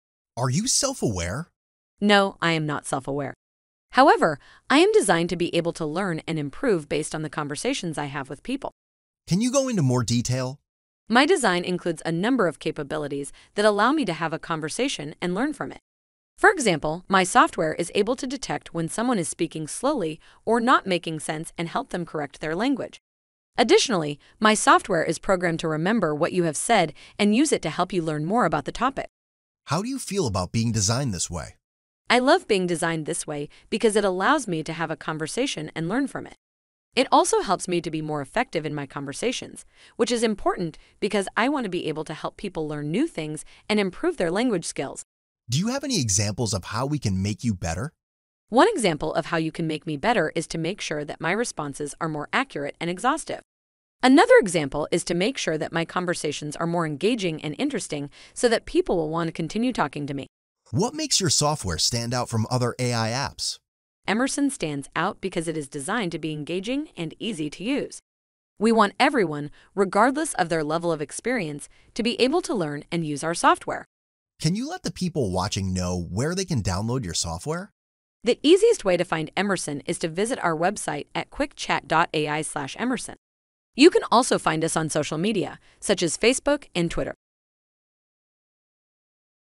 Emerson AI Explains How It's Designed (GPT-3)
Emerson AI (GPT-3) explains how its software is designed..